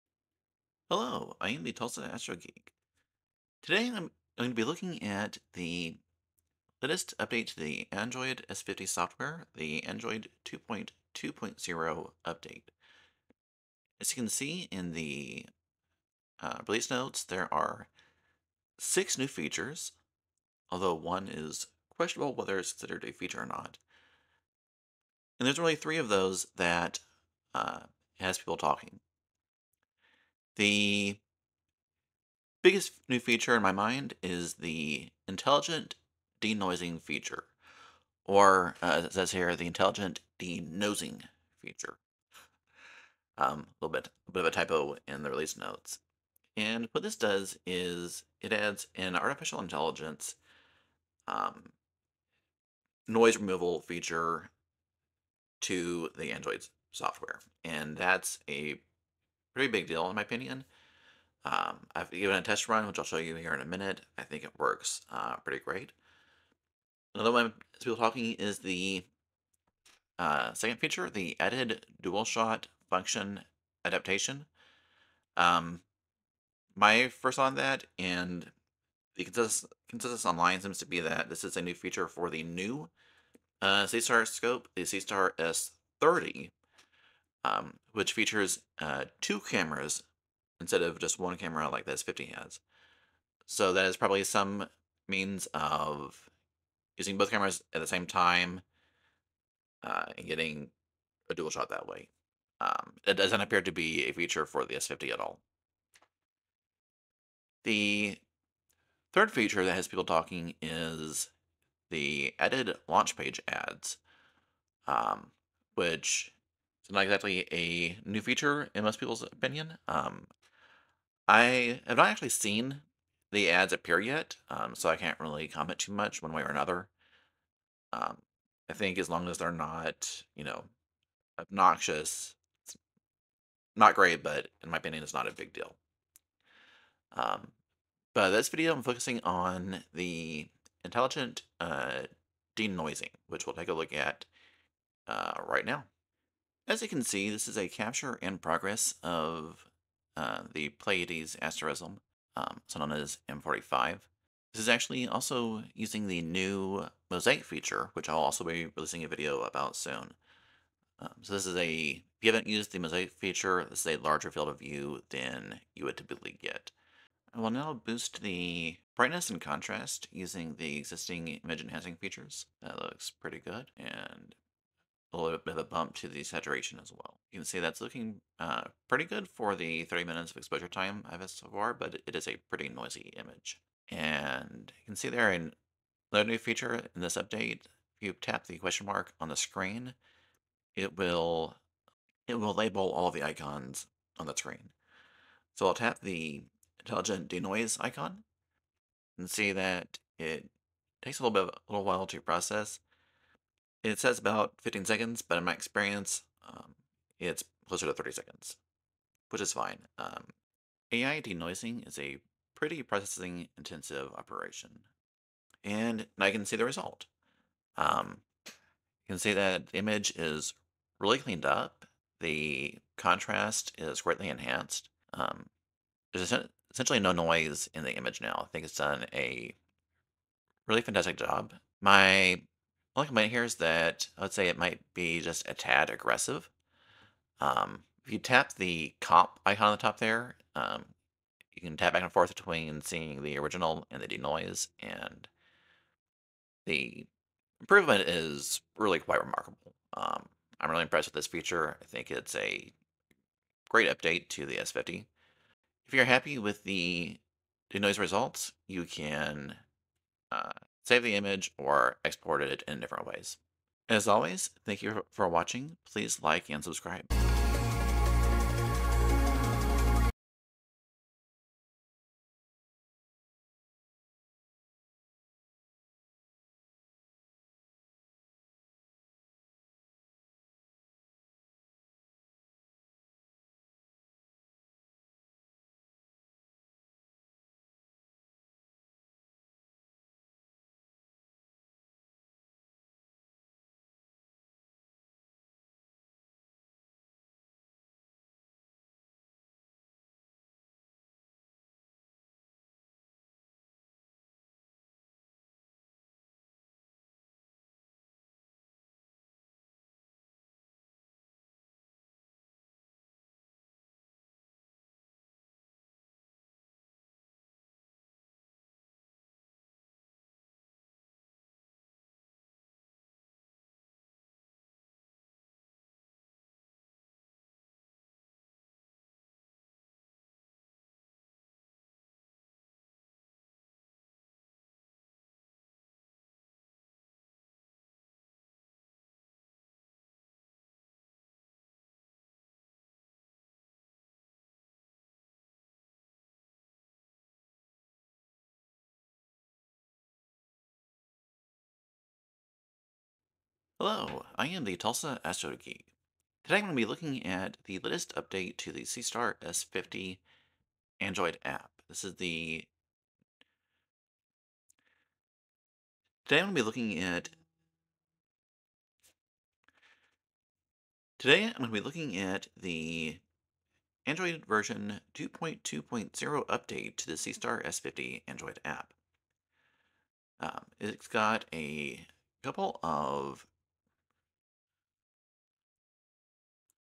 Is This AI Denoise Feature Too Good to be True? November 2024 Seestar update
In this video, we test out the AI denoise feature on the Seestar S50 with the November 2024 update. Is this feature too good to be true? Find out now!